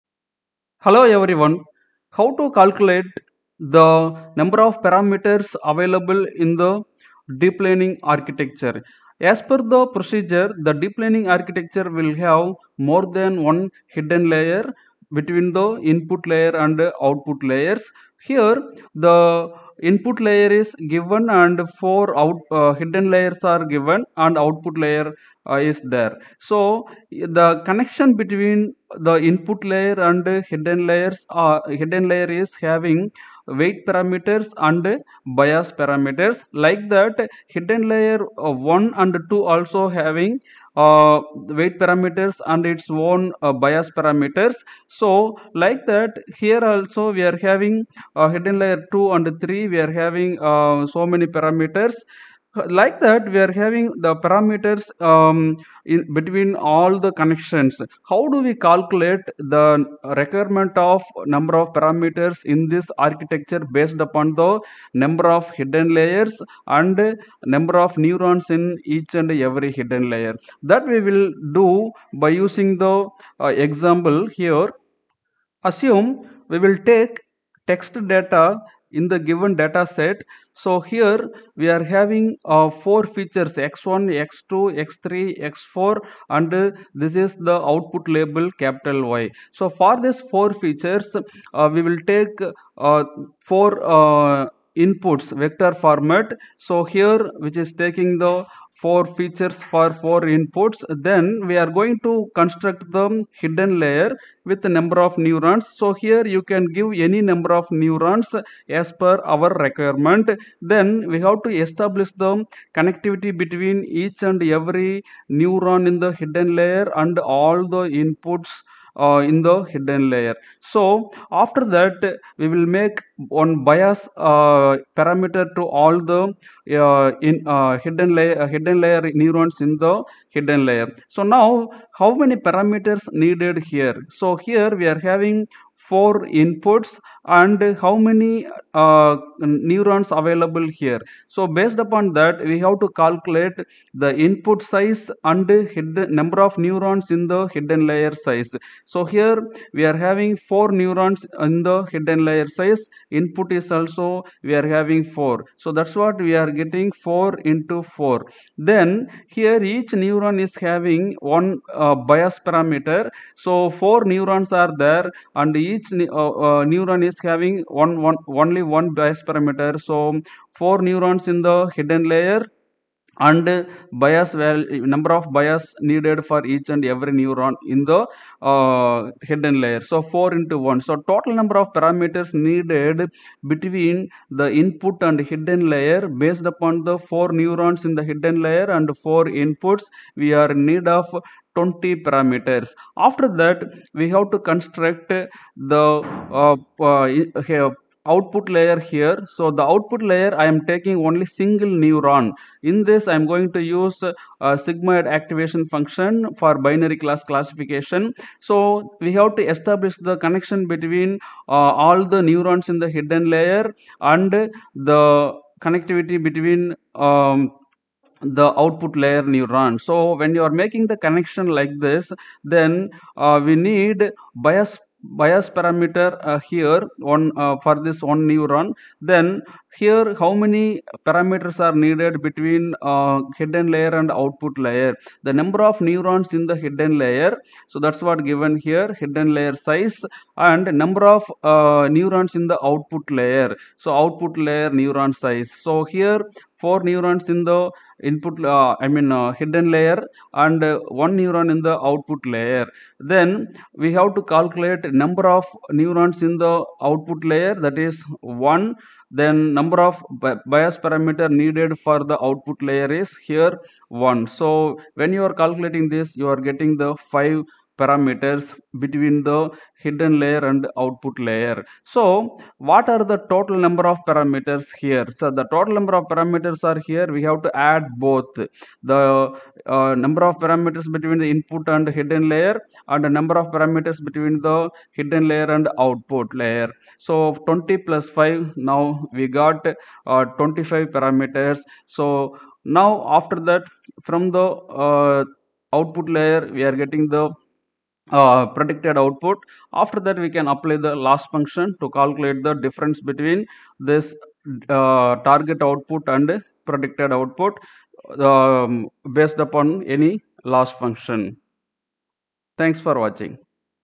07 CNN Forward Propagation - Parameters Calculation for Text or Grayscale Image input
Forward Propagation - Neural Network Parameters Calculation for Text or Grayscale Image input,
Bias and Weight Parameters